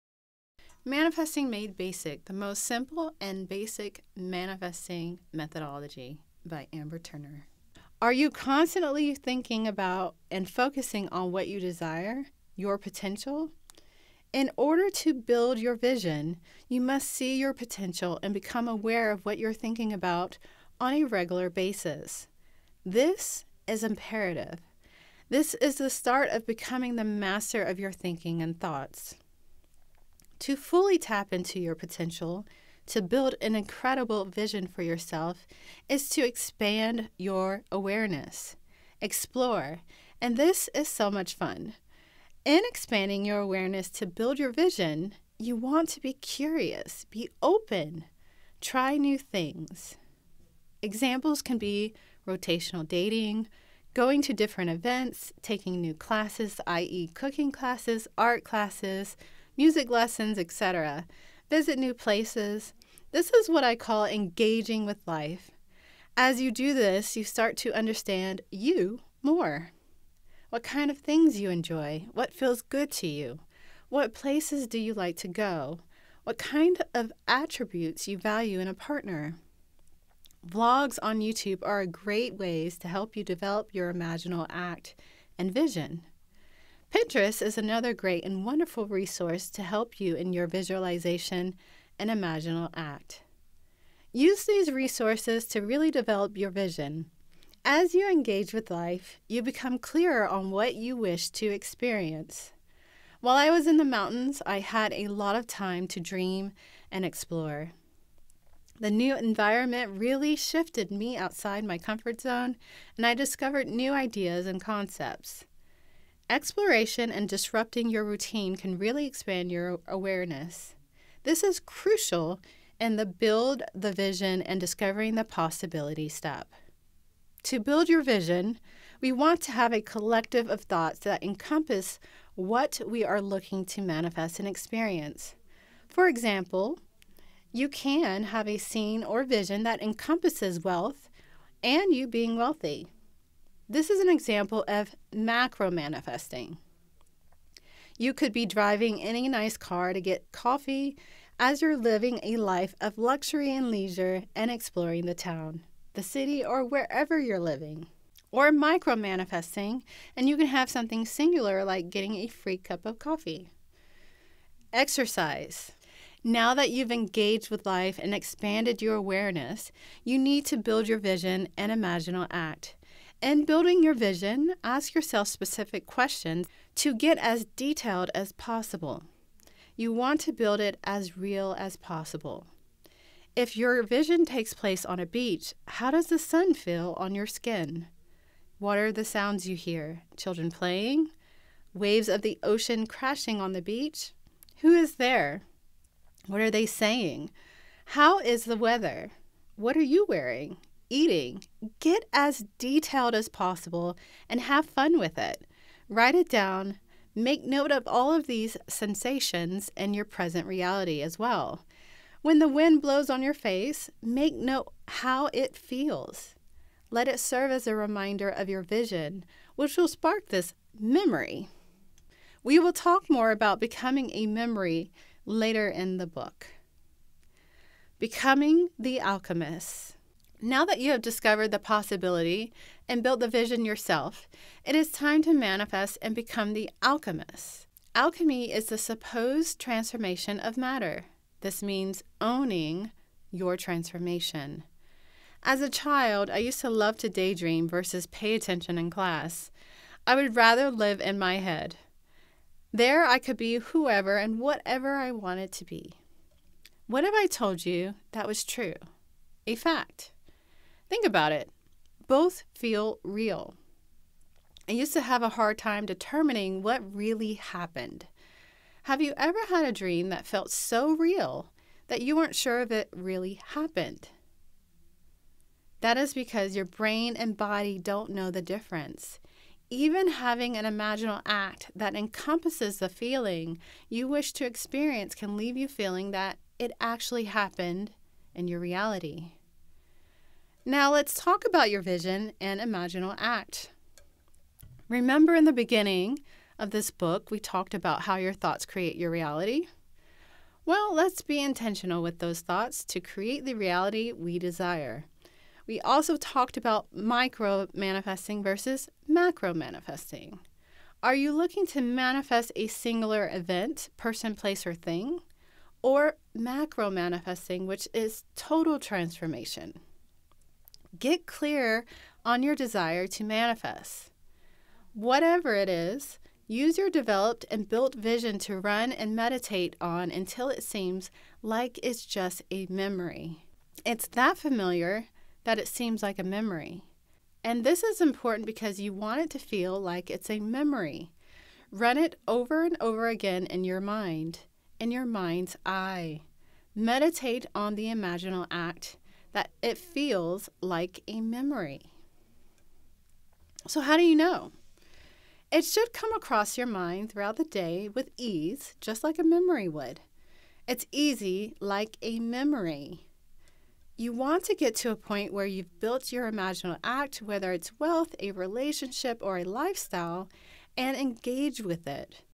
How To Discover Your Personal Potential ✨️ Part 2: Narrating Manifesting Made Basic
💫I have transformed my life intentionally by becoming clear on what I wanted and changing my thoughts.

❤I enjoy speaking about feminine energy and how it works in what I call "Power Coupling". Manifesting your dream relationship is possible!

💖Learn how to activate more Femininine Energy throughout your day with FADE (Fem Activation Daily Evaluation) Tool: